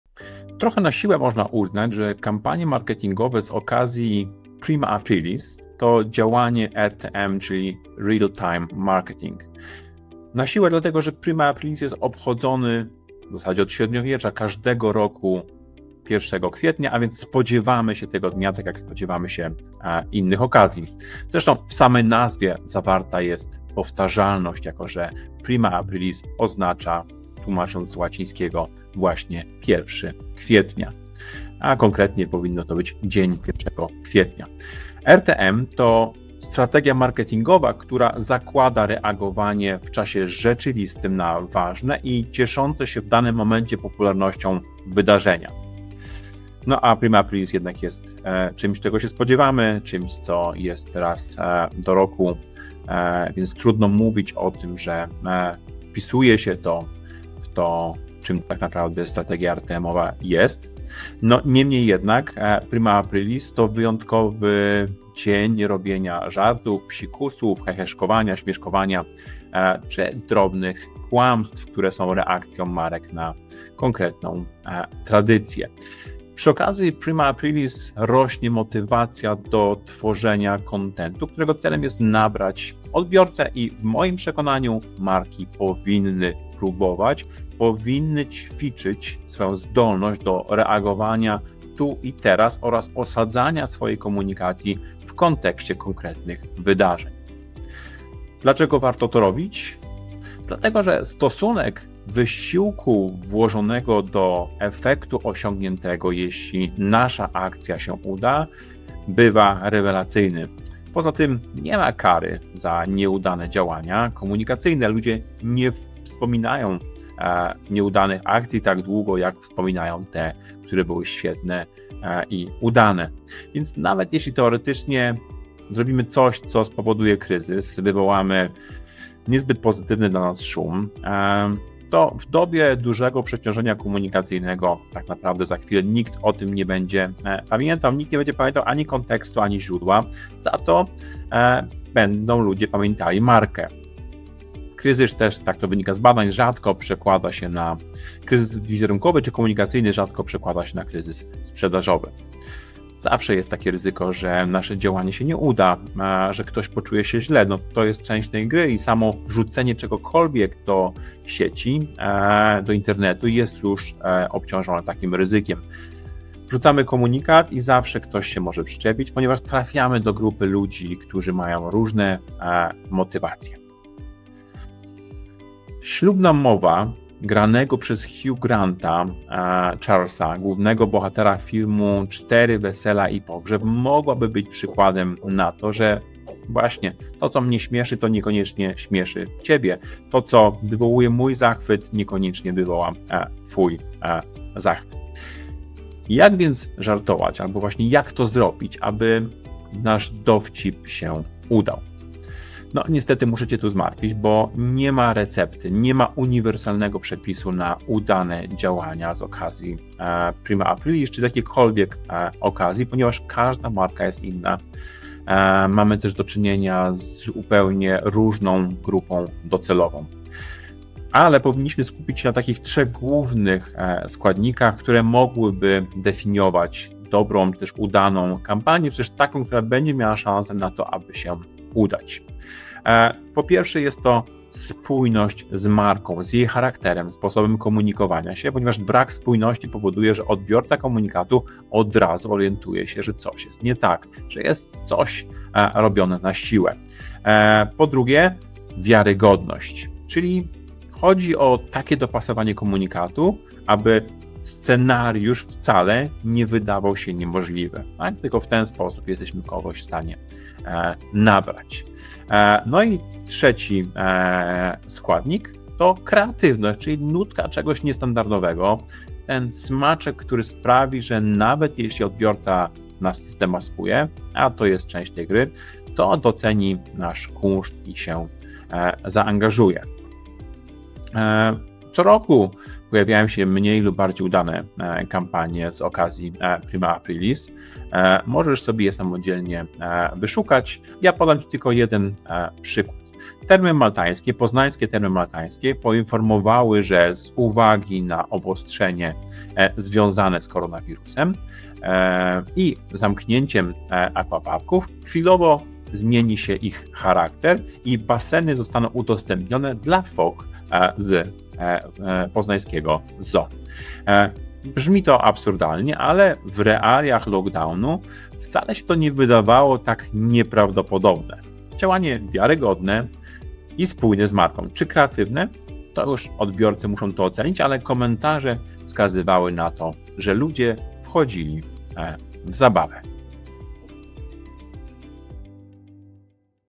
Prima aprilis w marketingu. Czy żart to dobra strategia?
🎉 Prima aprilis to nie tylko zwyczajowe święto żartów, ale także świetna okazja do kreatywnego real time marketingu!

🚀 Firmy na całym świecie wykorzystują ten dzień, aby zaskoczyć swoich klientów świeżymi i zabawnymi treściami. 😄 Od nietypowych promocji po zabawne memy - wszystko jest możliwe! 🌟 Pozostaje tylko jedno pytanie: czy z żartem można przesadzić? Jak długo kliencie wspominać będą humorystyczny niewypał, jeżeli taki nam się przytrafi? ☹

💥O wszystkim opowiada nasz uniwersytecki ekspert, specjalista od budowania marek – Łukasz Murawski.